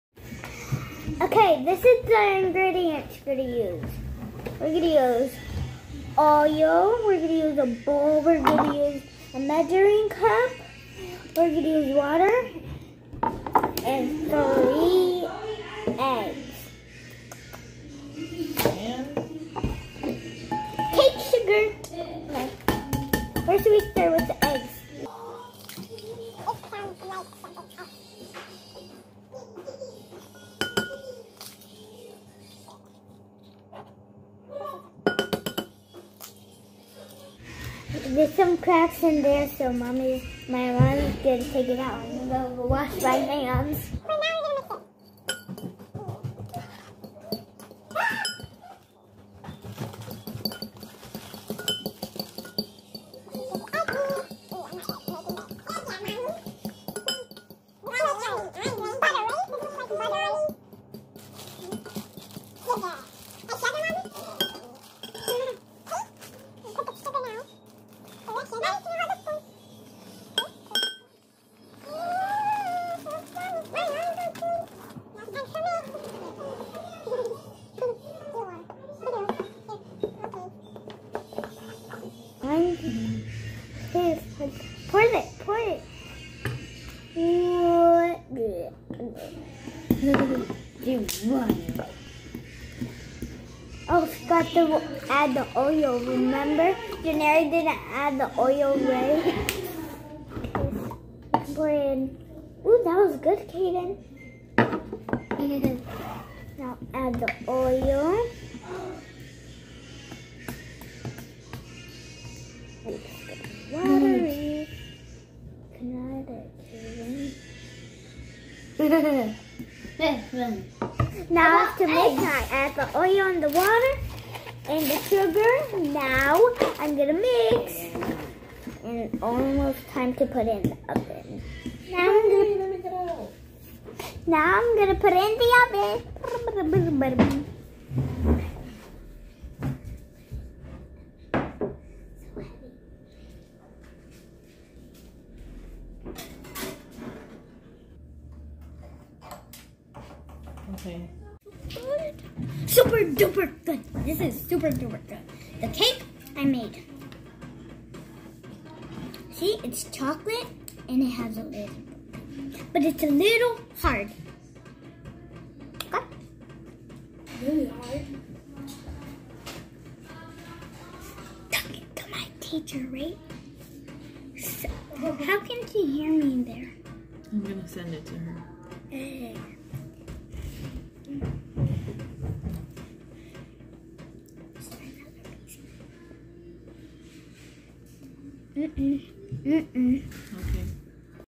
Bake with a 4 year old (chocolate 🎂)
My 4 year old love baking chocolate cake